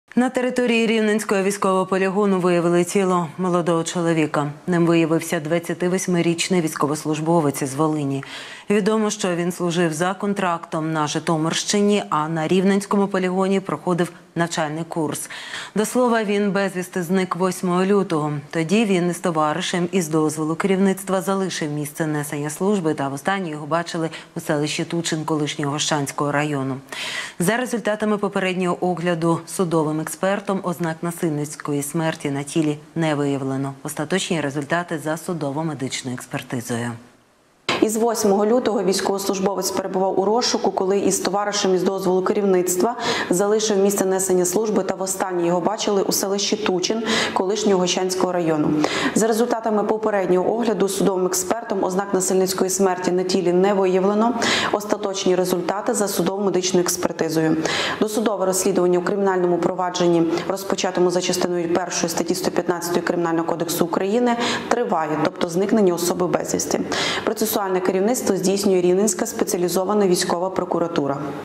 Військового з Волині, який зник безвісти, знайшли мертвим на полігоні
На території Рівненського військового полігону виявили тіло молодого чоловіка. Ним виявився 28-річний військовослужбовець з Волині. Відомо, що він служив за контрактом на Житомирщині, а на Рівненському полігоні — проходив навчальний курс. До слова, він безвісти зник 8 лютого. Тоді він із товаришем із дозволу керівництва залишив місце несення служби та востаннє його бачили у селищі Тучин колишнього Гощанського району. За результатами попереднього огляду судовим експертом — ознак насильницької смерті на тілі не виявлено. Остаточні результати — за судово-медичною експертизою.